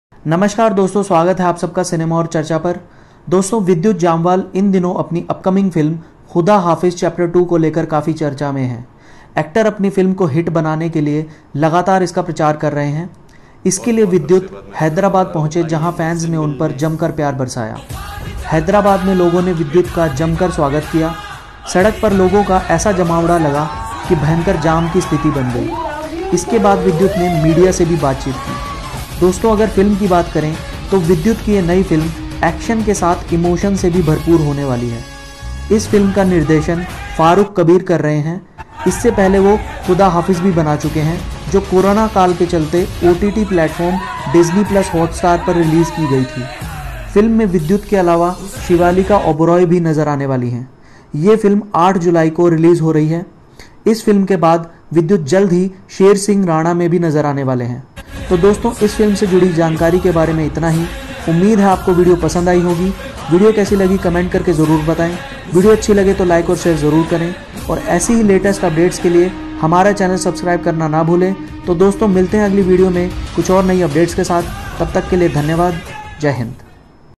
Vidyut Jamwal in Hyderabad 💫| Khuda Haafiz 2 promotions💯|विद्युत ख़ुदा हाफ़िज़ 2 के लिए हैदराबाद में
The video contains the latest update of the vidyut jamwal upcoming movie khuda Hafiz 2.

@cinema aur charcha